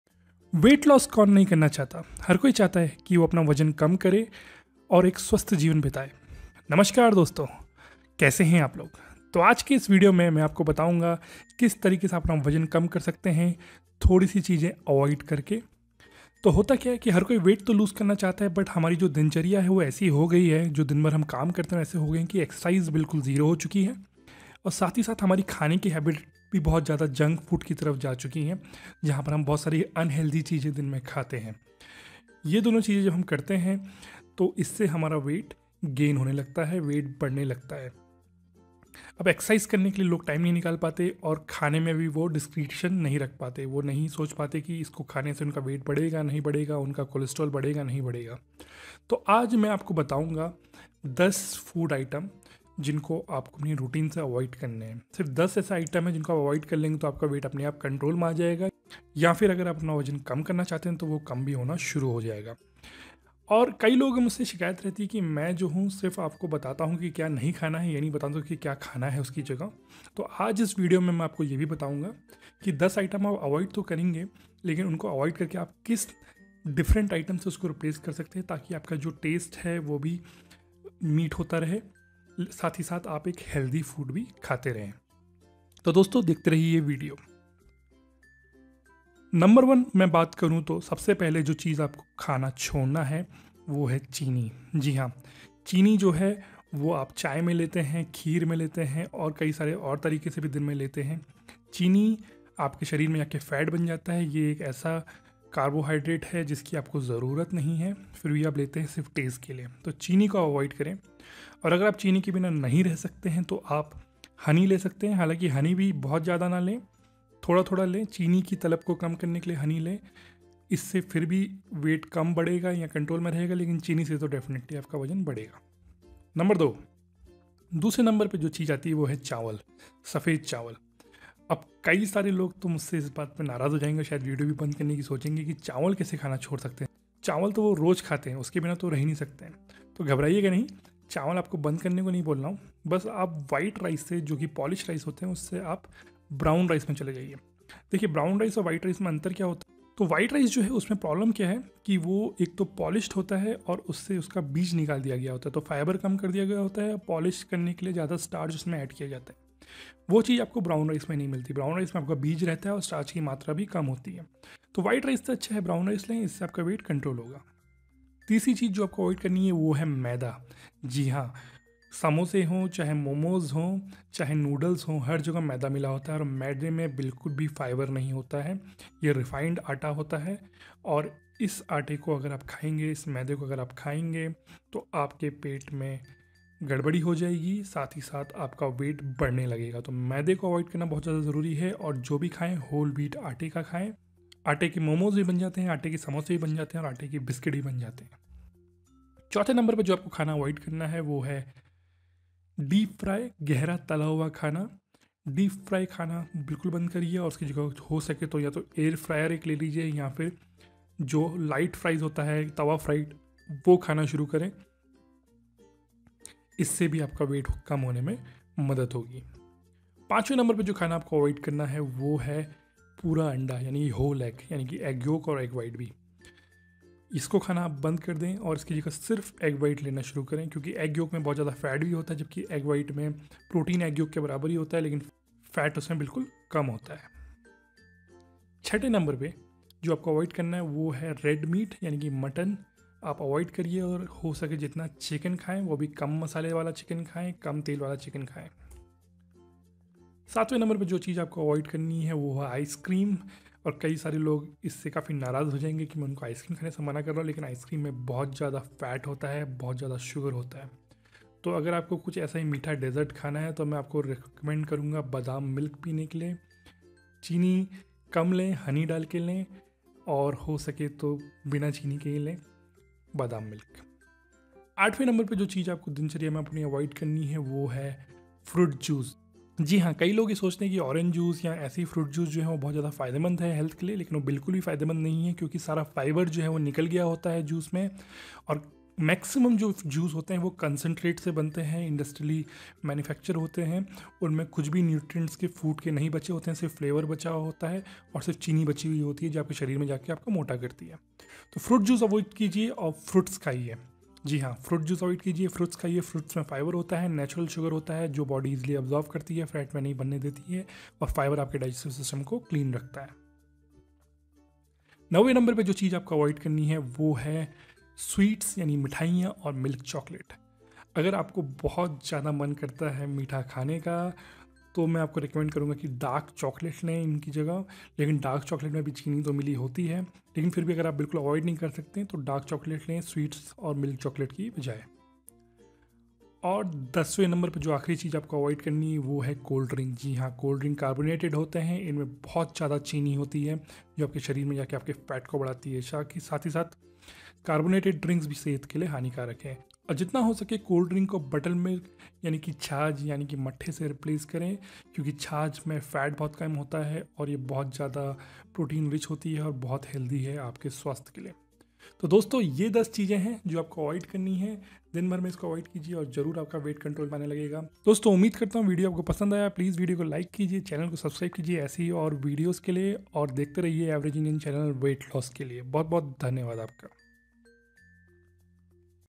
वजन कम करना है तो ये बिलकुल नहीं खाये! (10 Food to AVOID for Weight Loss )
Vajan kam karne ke liye kya khana chahiye?

Weight loss tips and food to AVOID for Weight Loss.

Weight loss can be achieved by eating low calories weight loss food and avoiding high calorie foods like sugar, rice etc. Watch this video to know what you should eat and what you should not to lose weight naturally. In this video we tell you 10 high calories food to avoid and replace them with 10 low calorie food.